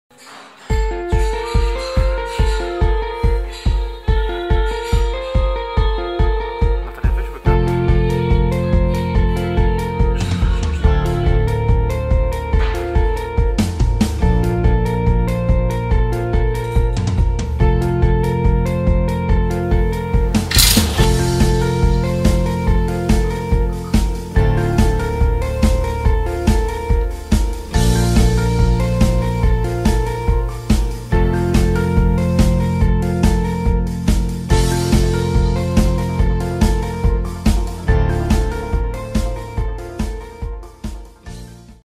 Słabe oświetlenie
OLYMPUS Om-D E-M 5 mk II Obiektywy f1.8 i f2.8 w słabych warunkach oświetleniowych.